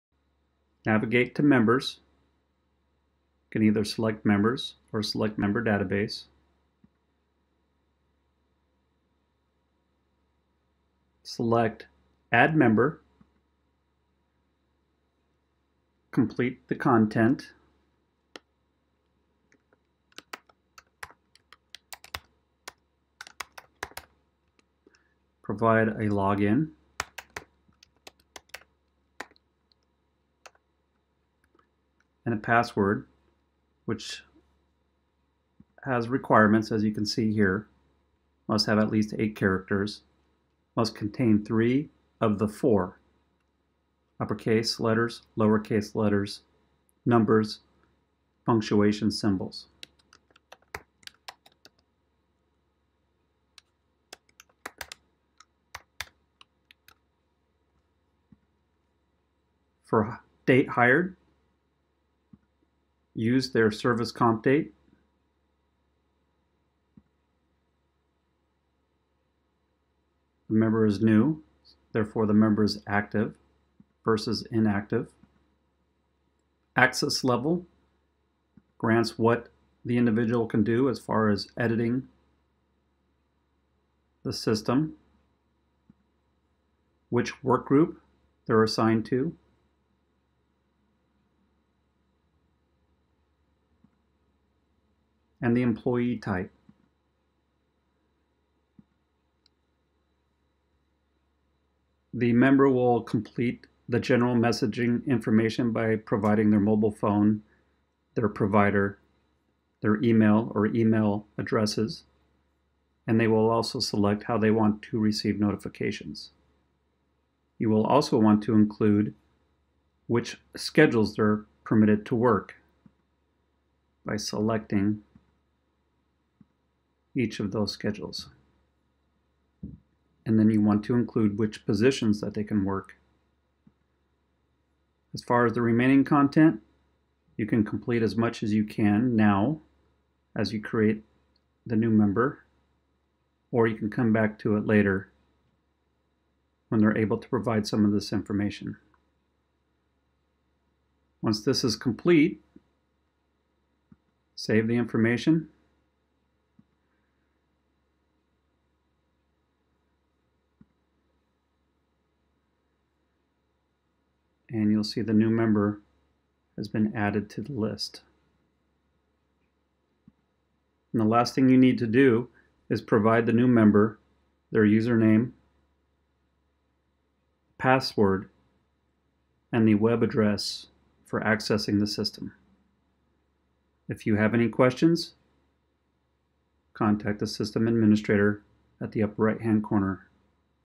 Aladtec: Adding New Members
Schedulers:  Describes how to add a new member in Aladtec - the online scheduling system used by NRNW F&ES.